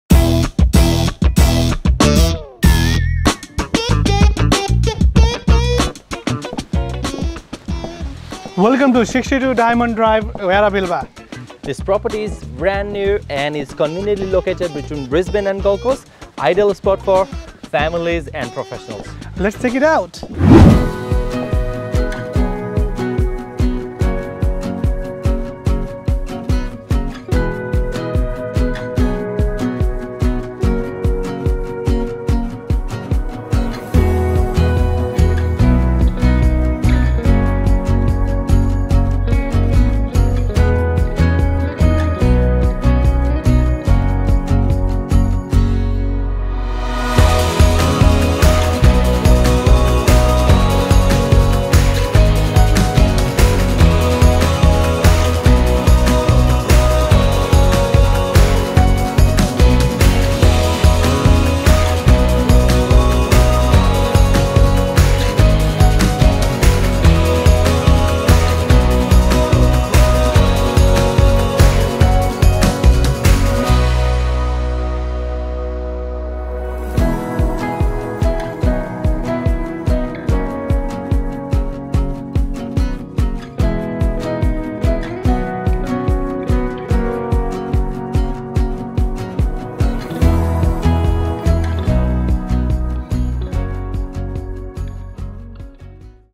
Sapphrie 62 Diamond Dr Yarrabilba
Welcome to 62 Diamond Dr, Yarrabilba! 📍

Situated perfectly between Brisbane and the Gold Coast, this property offers the ideal blend of convenience and comfort. With top schools, gyms, and shopping centres just a stone’s throw away, it’s the perfect spot for both families and professionals.

Take a tour and see why this could be your next home!